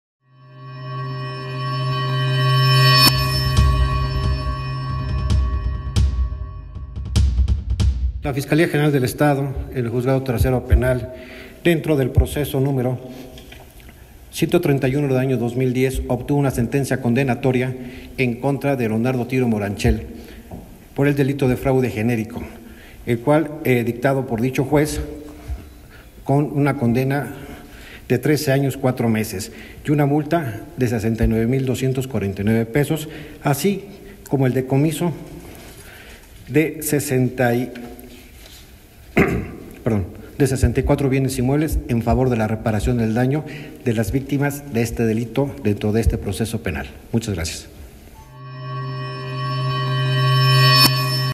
Dictan 13 años de prisión a Leonardo Tiro Moranchel por fraude de Grupo Sitma
La Fiscalía General del Estado de Puebla, dio a conocer la sentencia de 13 años de prisión a Leonardo Tiro Moranchel, por el delito de fraude genérico a inversionistas del Grupo Sitma en el año 2009.
28 enero 2019